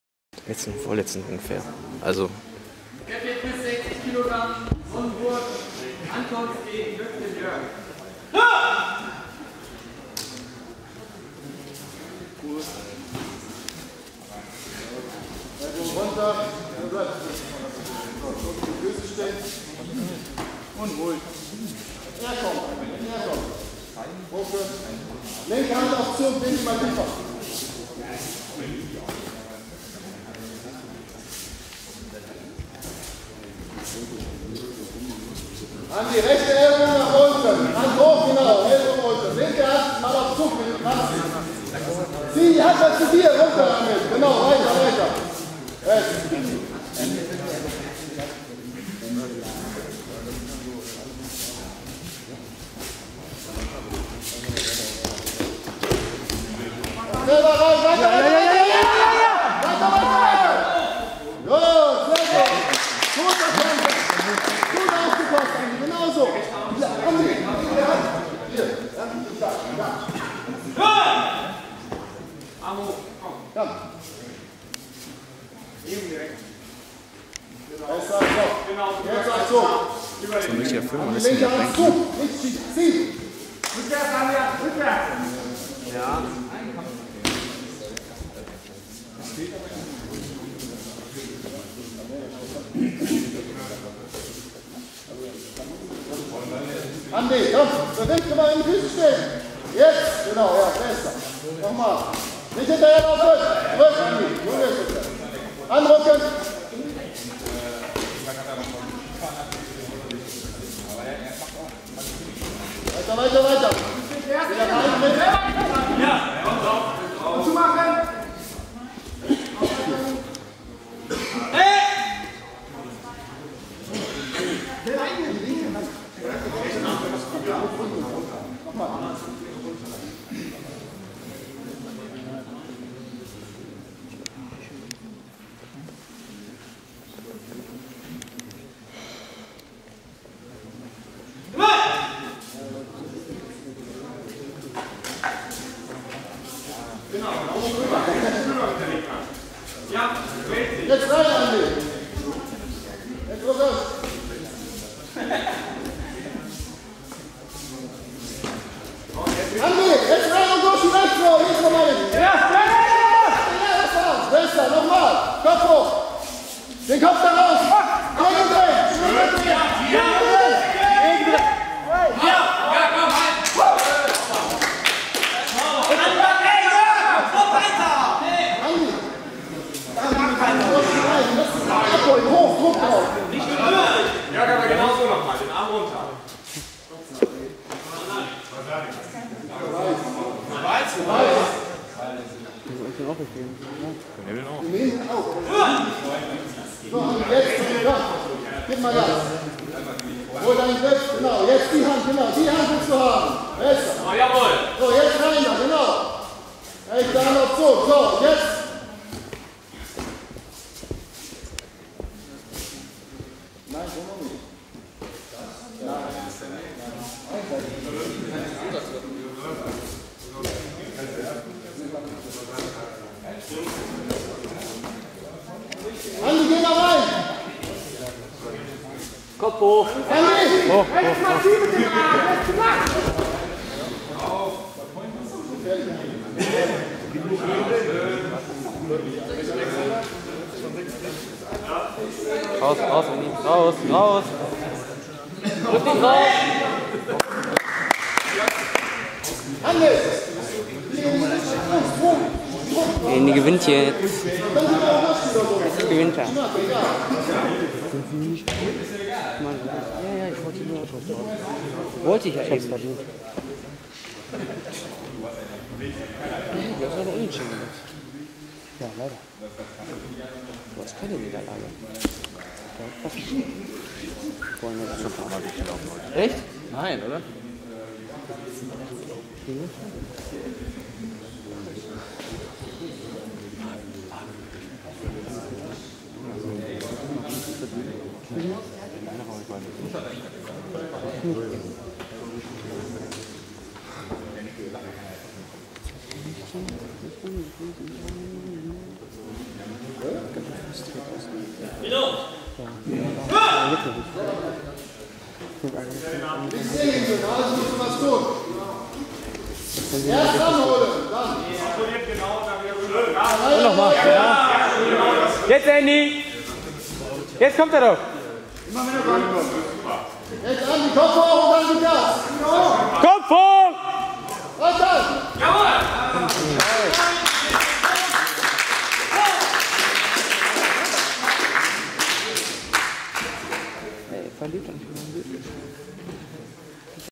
Tus Lendringsen vs. TV Attendorn [-60]
Fünfter Kampftag der Bezirksliga Arnsberg 2012
Für Lendringsen kämpft Andreas [rot]